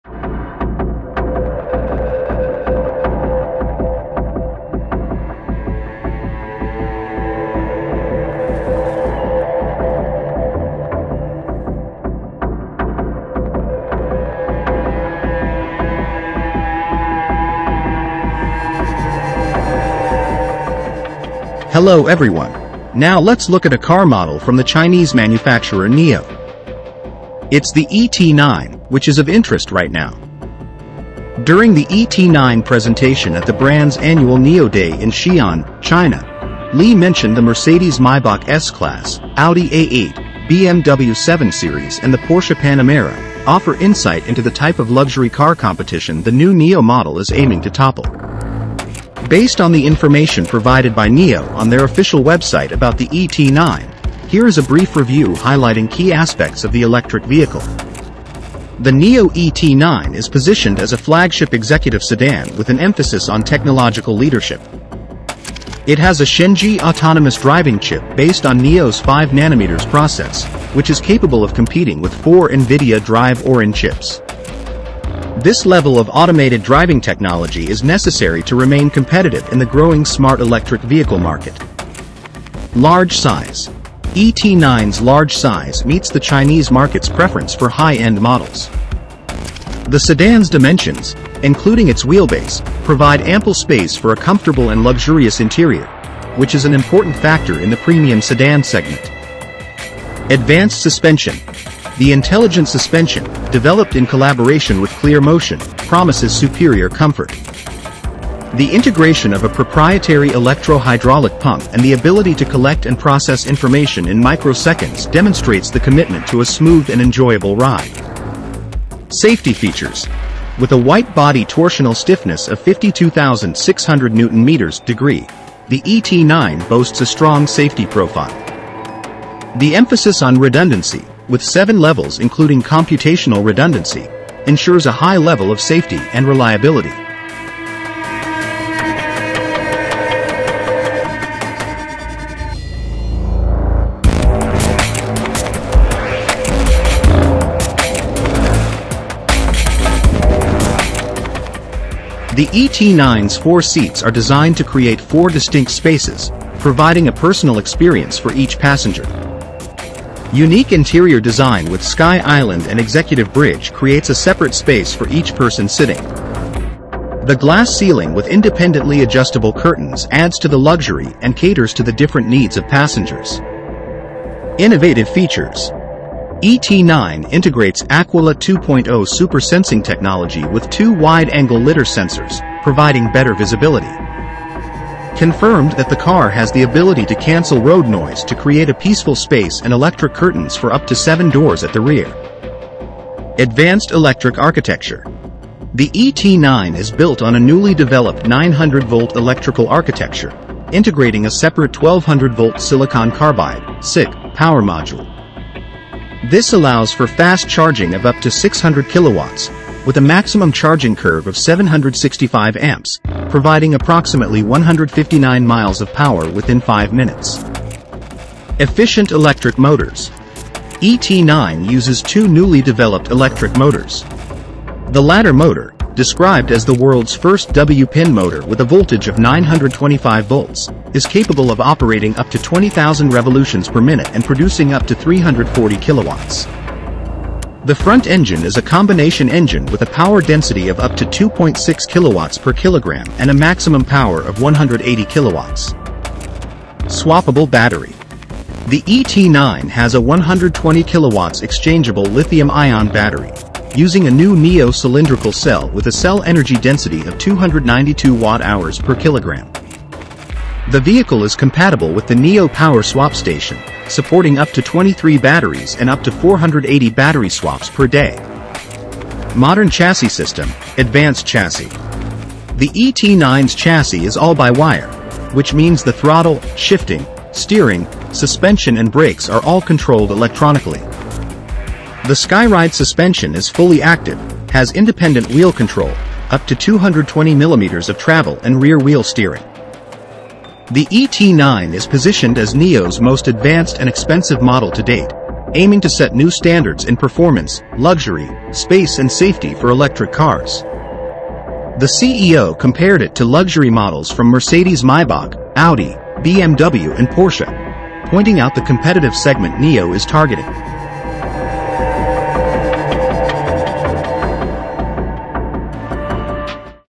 Nio ET9 Electric 900 volts, 600Kw Fast Charging in 5 Minutes and 159 Miles. Rival Porsche Panamera.
Nio ET9 Electric Vehicle Model 900 volts, 600Kw Fast Charging in 5 Minutes and 159 Miles. Advanced Electric Architecture:
The ET9 is built on a newly developed 900-volt electrical architecture, integrating a separate 1200-volt silicon carbide (SIC) power module. This allows for fast charging of up to 600 kW, with a maximum charging curve of 765 amps, providing approximately 159 miles of power within 5 minutes.